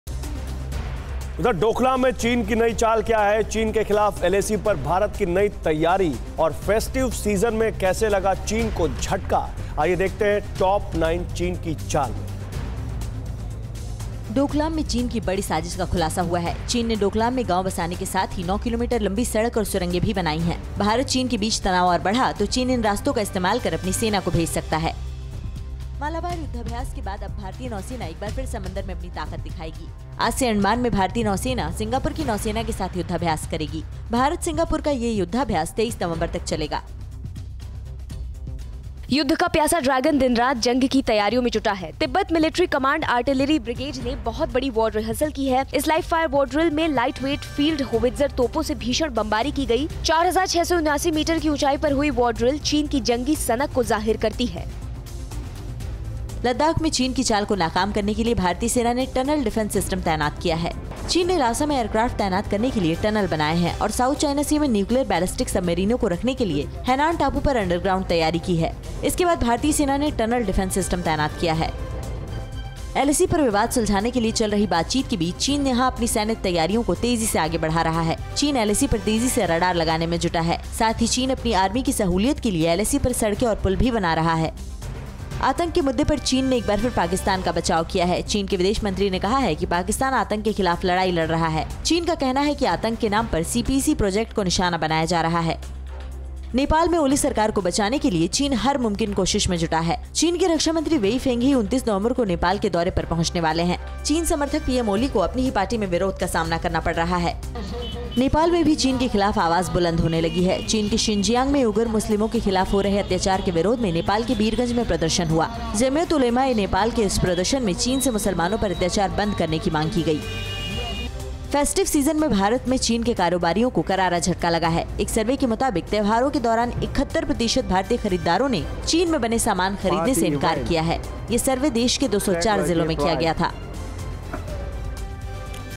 Top 9 चीन की चाल: भारत में चीन के कारोबारियों को झटका, 71% भारतीयों ने Chinese Product को कहा ना
-डोकलाम में चीन की बड़ी साजिश का खुलासा
-युद्ध का प्यासा ड्रैगन दिन रात जंग की तैयारियों में जुटा
-आतंक के मुद्दे पर चीन ने एक बार फिर पाकिस्तान का बचाव किया है